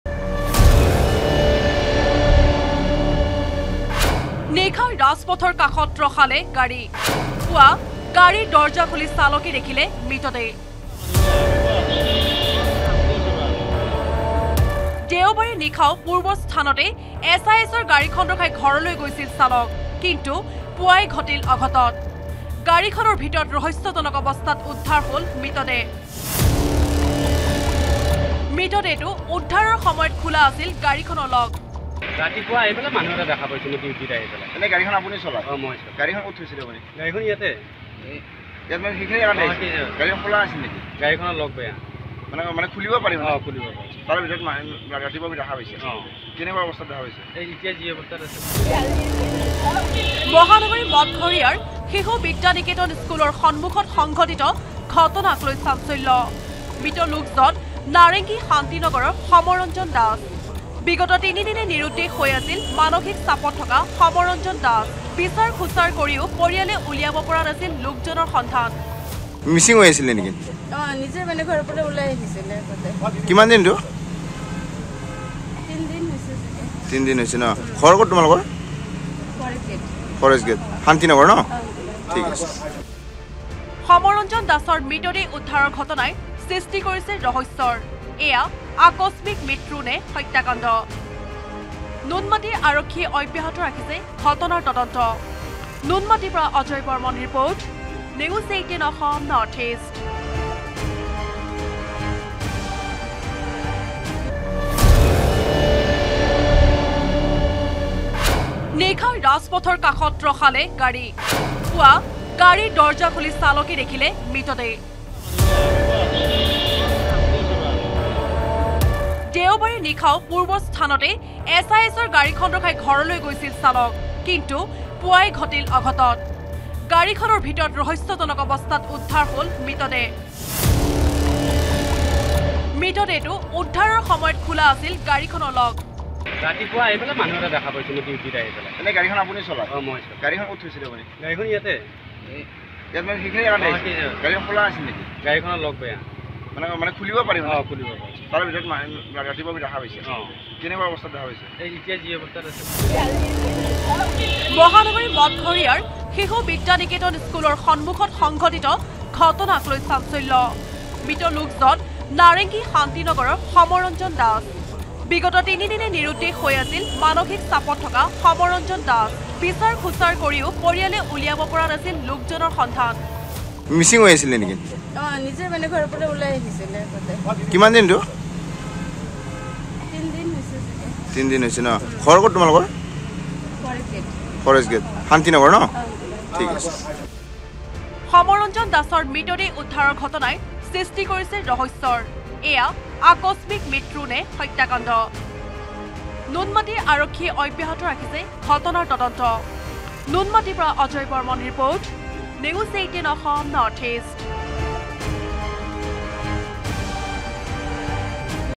Guwahati Incident : পুৱা গাড়ীৰ দৰ্জা খুলি চালকে দেখিলে মৃতদেহ | ক'ত কিয় ঘটিল এই অঘটন ? N18V
Guwahati Incident | মহানগৰীৰ মঠঘৰীয়াত মৃতদেহ উদ্ধাৰ।
এখন বাহনৰ ভিতৰত উদ্ধাৰ মৃতদেহটো।
শিশু বিদ্যা নিকেতনৰ সন্মুখত উদ্ধাৰ মৃতদেহটো।
মৃত লোকজন নাৰেংগী শান্তি নগৰৰ সমৰ ৰঞ্জন দাস।
বাহনখনৰ চালকে প্ৰত্যক্ষ কৰে মৃতদেহটো।
তিনিদিন ধৰি সন্ধানহীন হৈ আছিল লোকজন।
দীৰ্ঘদিনে মানসিক ৰোগত ভুগি আছিল সমৰ ৰঞ্জন দাস।
n18oc_crime